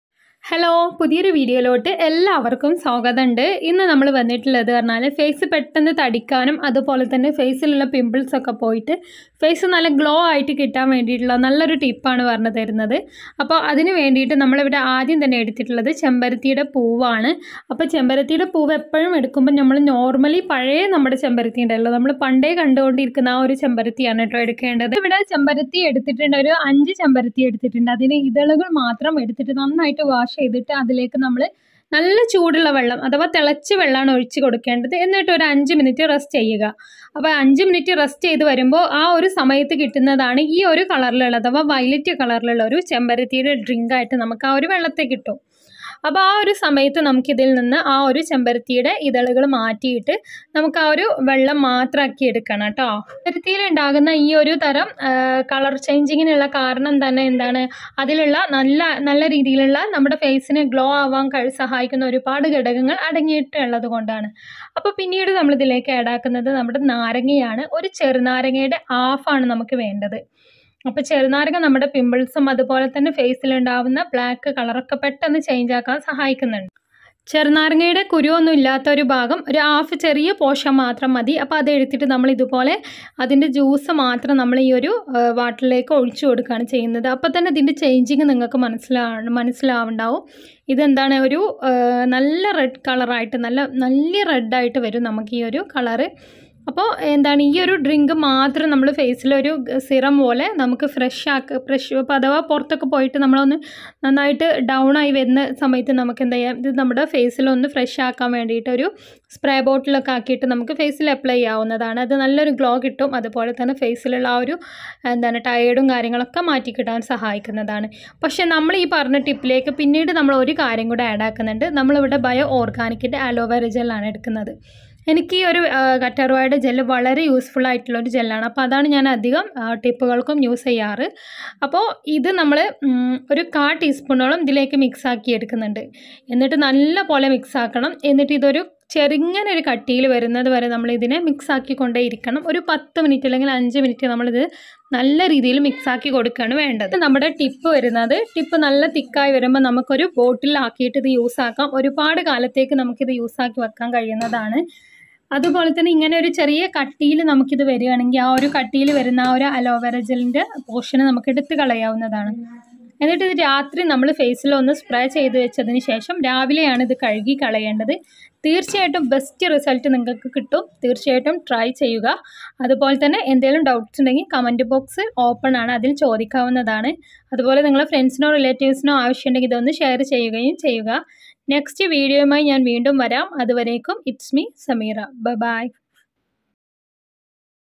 മുഖം തുടിക്കാനും നിറം വെക്കാനും ഇത് രാത്രിയിൽ പുരട്ടി നോക്കൂ രാവിലെ കാണാം മാജിക്ക് |cheeks whiting
കൂടുതൽ അറിയാൻ വീഡിയോ മുഴുവനായും കാണാൻ മറക്കല്ലേ👉🏽😊😊

Weight loss
Weight gain tips
Weight gain chart
Ottamoolikal
Shareera vedanakk
Mukham niram vekkan
Neer pokaan
Eethapazham kazhichal